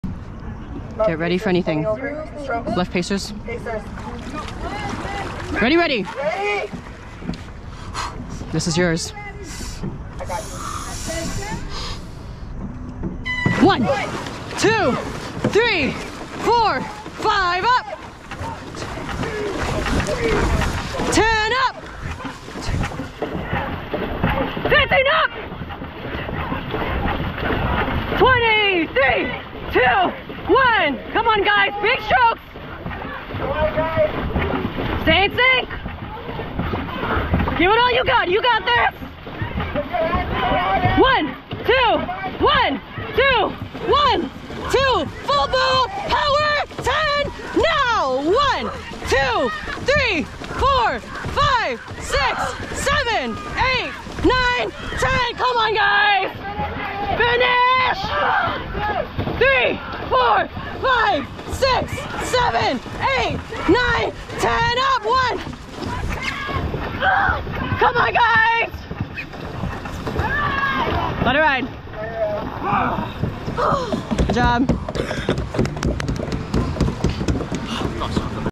QC Cup 2023 - BANANASWIFT - 200m Heat
QC Cup 2023
BANANASWIFT - 200m Heat
Result: 6 out of 6 - 00:55:92

Timestamps:
00:00 Line up
00:11 Start
02:15 Finish